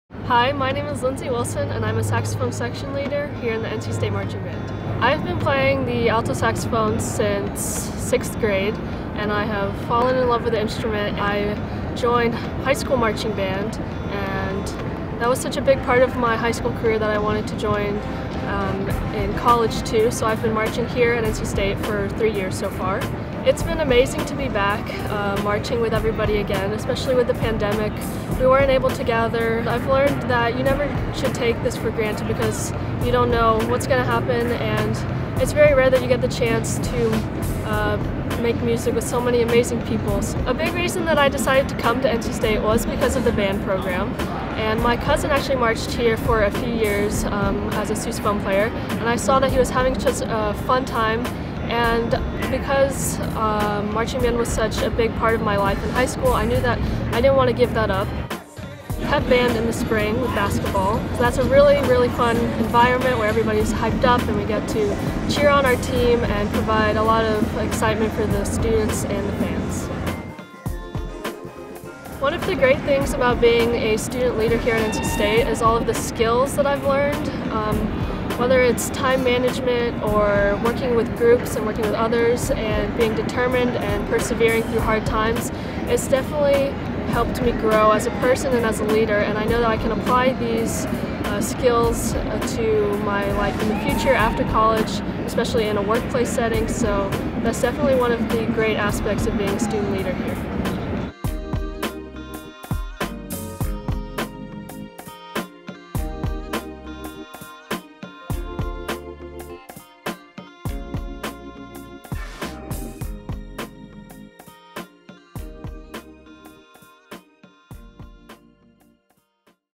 Student Spotlight: Lindsey Wilson
Junior Lindsey Wilson is a saxophone section leader in the marching band, a member of the varsity pep band and a recipient of the Alby Rose Marching Band Scholarship. She's majoring in biology and minoring in genetics and environmental sciences. This is her story of serving in a leadership role with the Power Sound of the South.